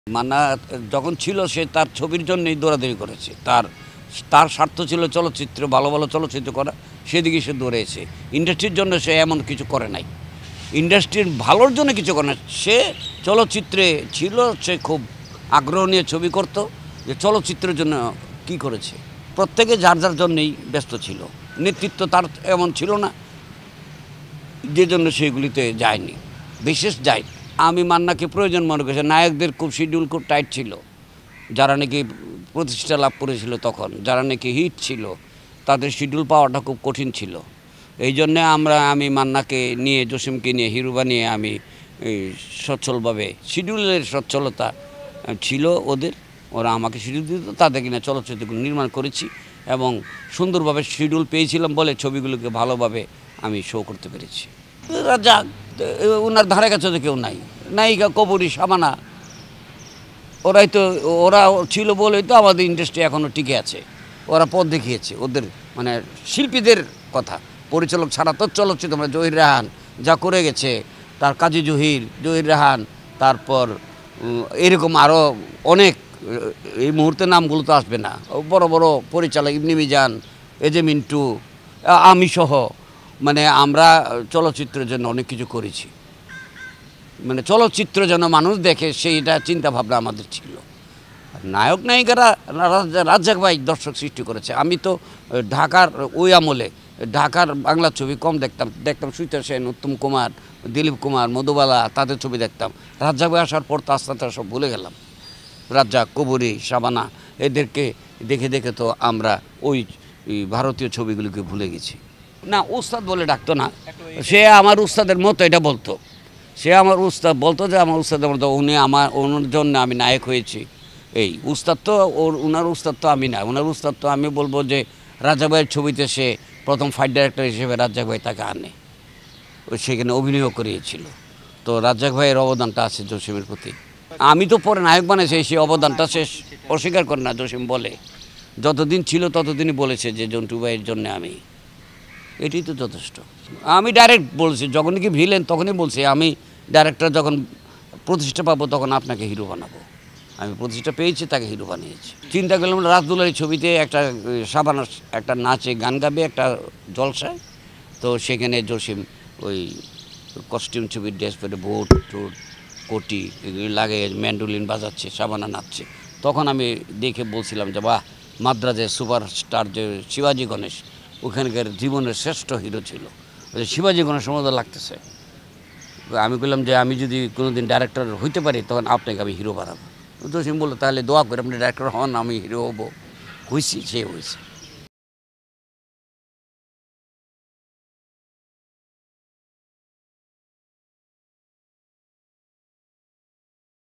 জসিমকে আমিই নায়ক বানিয়েছি | Delwar Jahan Jhantu | Film Director | Manna | Janakantha
মান্না যা করেছে নিজের জন্য, ইন্ডাস্ট্রির ভালোর জন্য করেনি: দেলোয়ার জাহান ঝন্টু
Connect with Janakantha Digital!